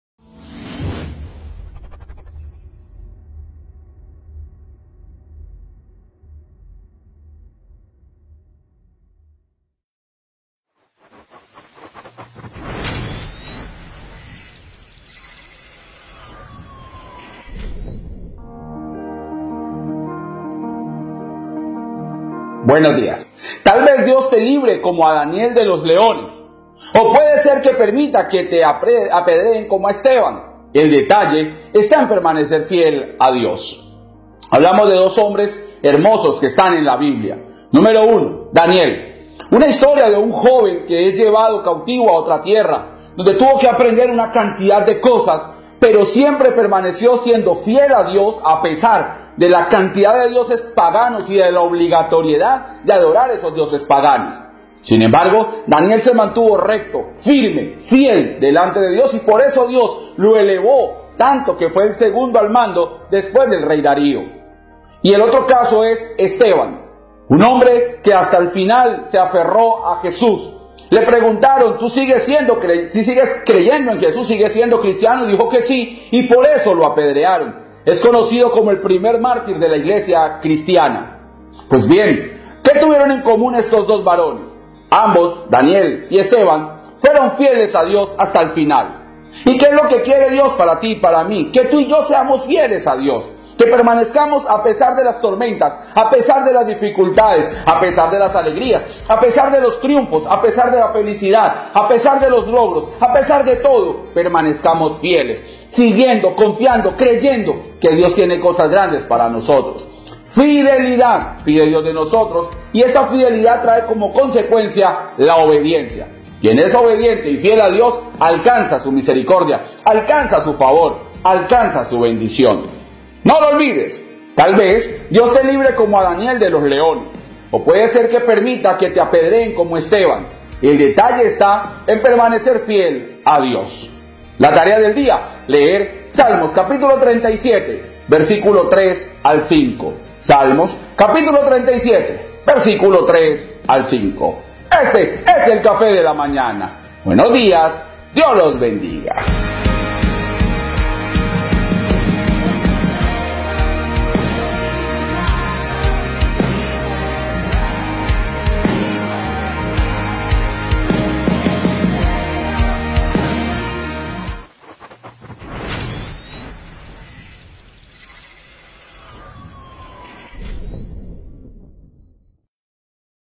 Cafe de la Mañana : Mantente fiel en el Señor y el te recompensará.
2017
Confía en el Señor y haz el bien establécete en la tierra y manténte fiel. Deléitate en el Señor y Él te concederá los deseos de tu corazón. Encomienda al Señor tu camino, confía en Él y Él actuará (Salmos:37:3-5)